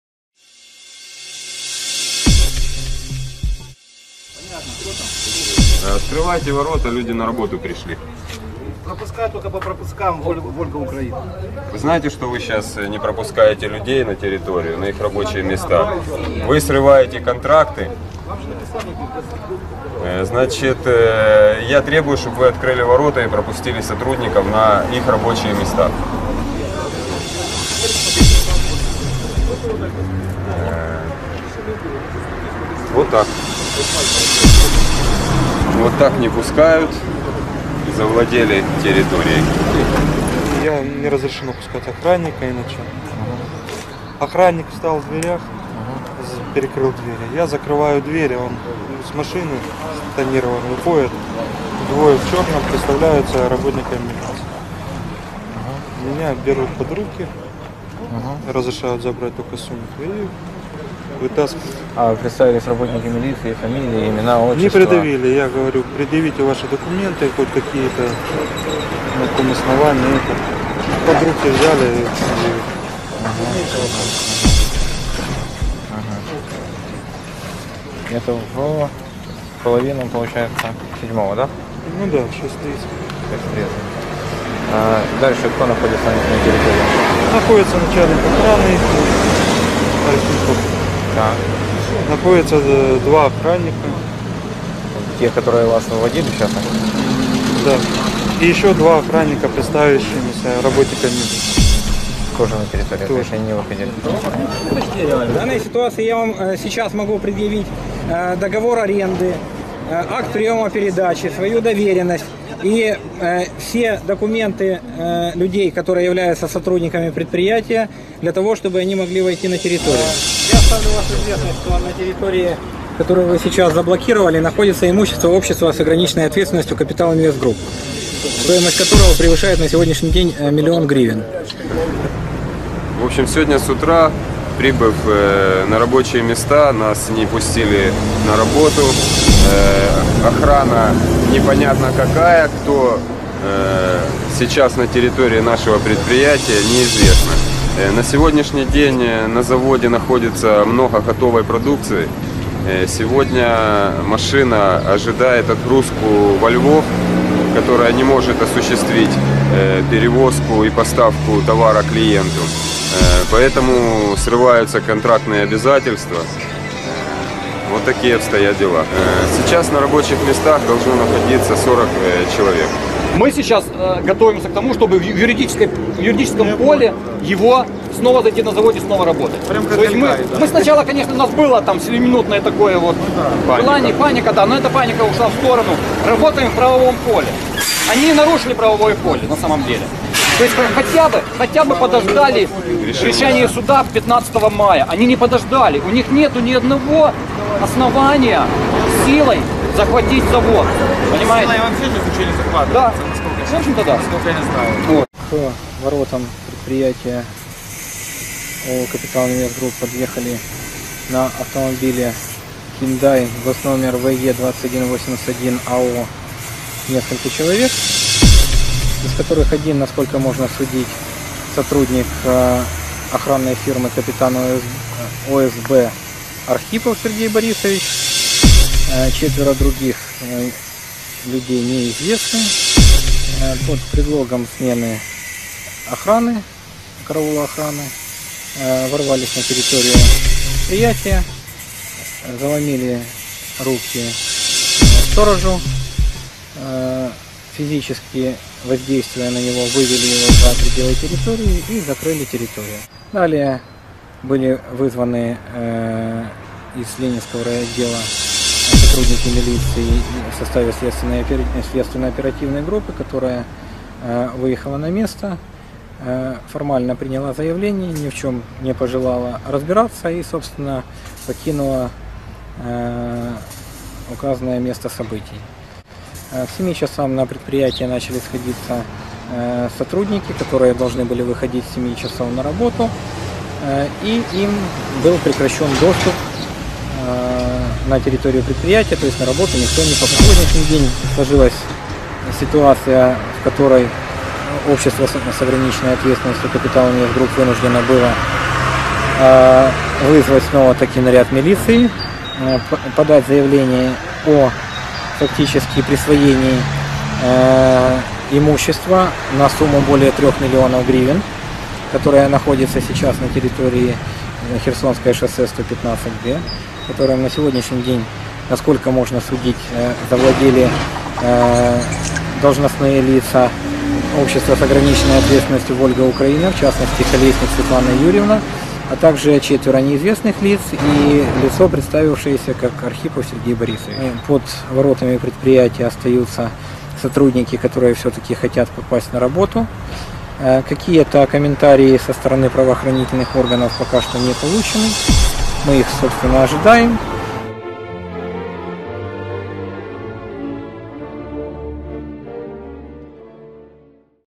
Противостояние на заводе УМК
За право контроля над заводом развернулась настоящая война между предприятиями «УМК» и «Вольга-Украина». В настоящее время вопрос о том, кто же имеет право находиться и работать на заводе, решается в судах.